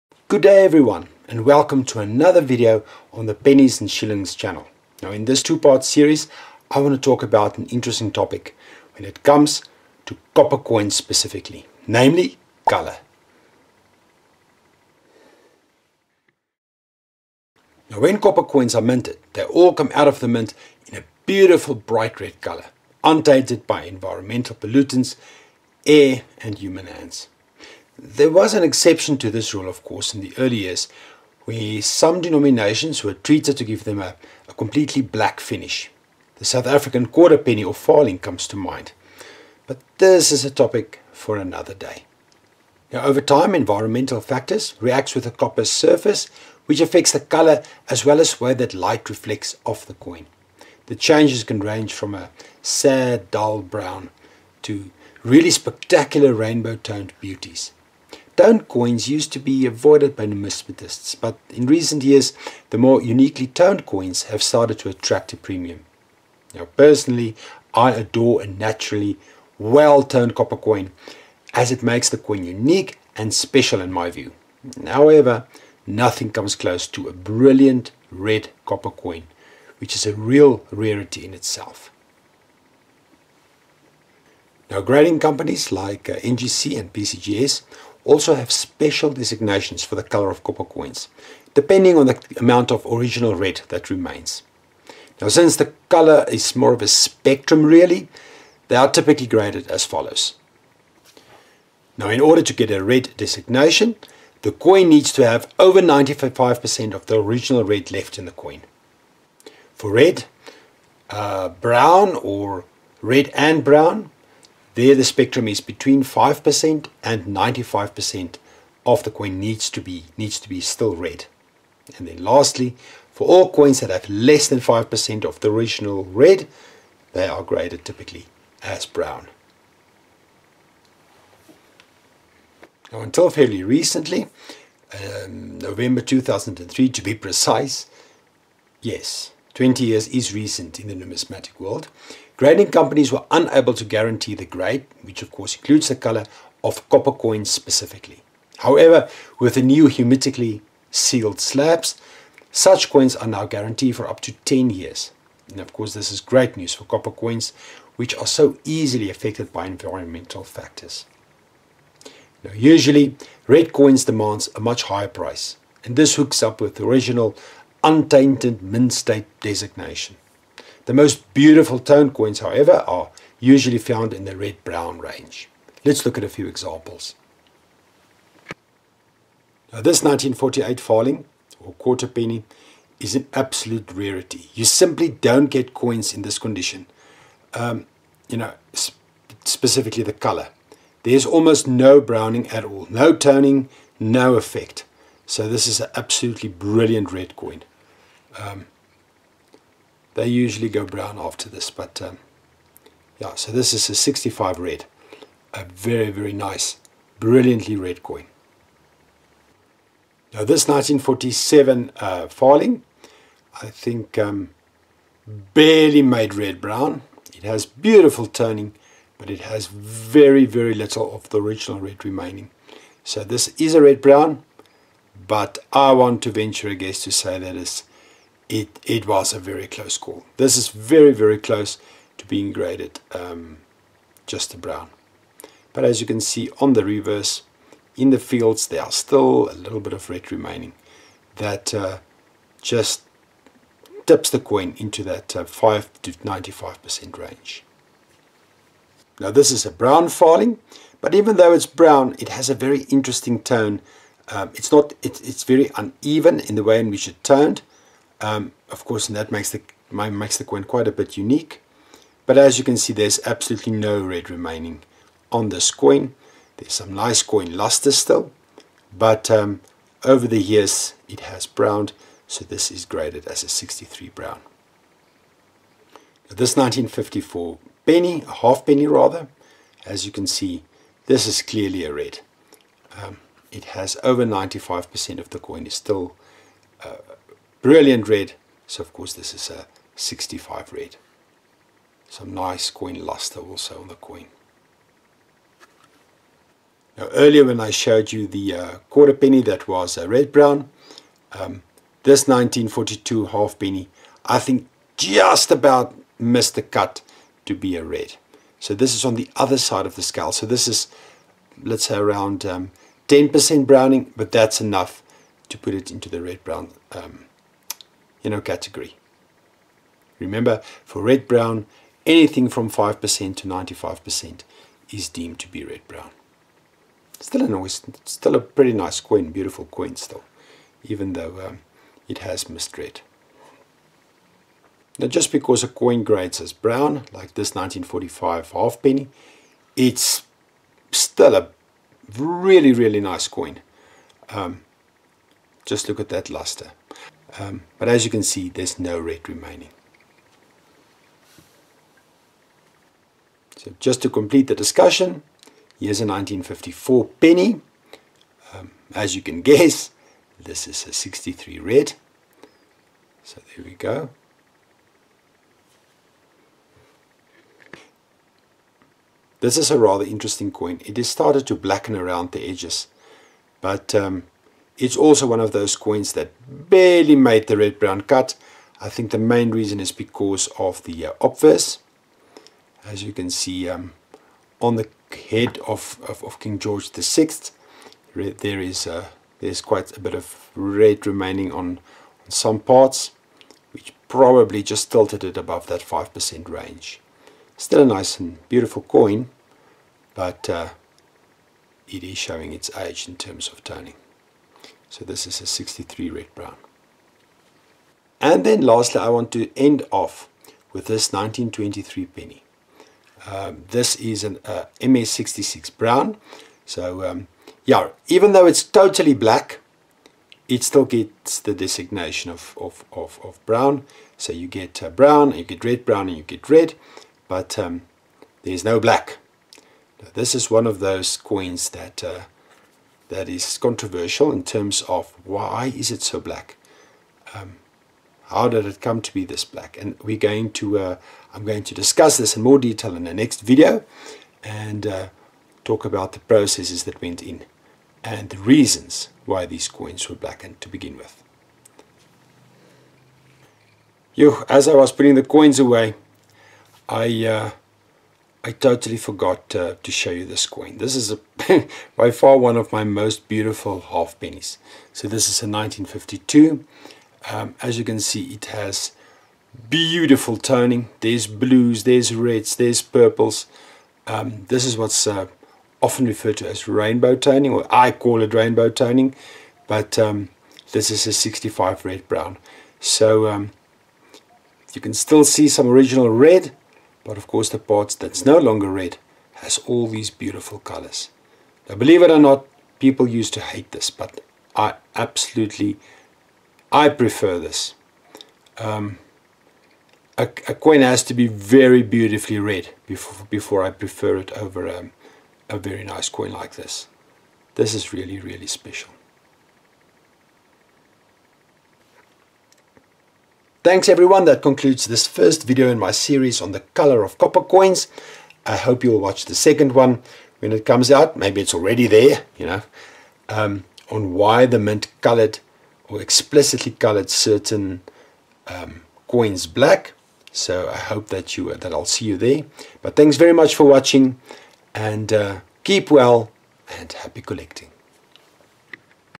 Where do copper coins get all their colours from?
Copper is a highly reactive metal and binds readily with various compounds in the atmosphere.  These reactions are responsible for not only causing the dull brown or pitch black but also the beautiful rainbow tones we often see on copper coins.  The colours are so intrinsic of copper coins that grading companies assign special colour designations to these coins as well, namely red, red-brown and brown.  It all depends on the amount of red that still remains on the coin.  The video concludes with some samples of coins with different colours and the grade received by NGC.